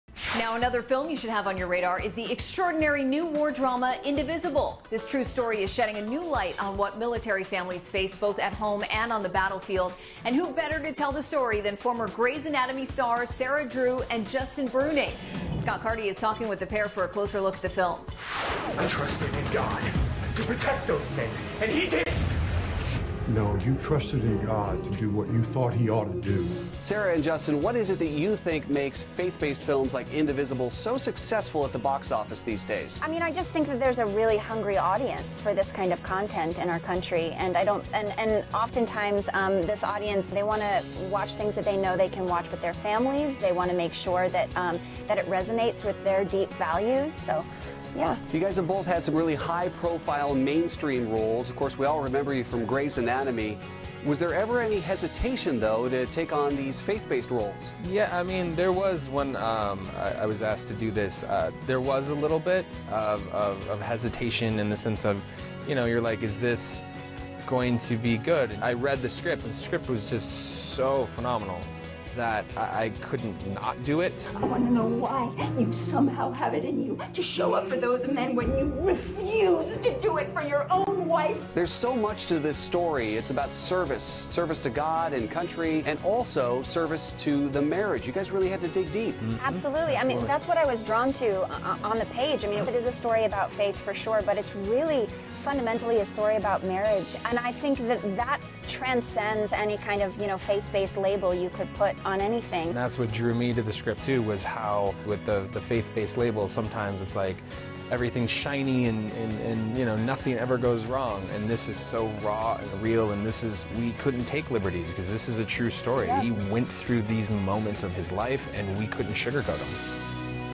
Indivisible: Closer Look | Celebrity Page
CP Correspondent Scott Carty talks with the cast of the new drama, 'Indivisible', starring Sarah Drew and Justin Bruening!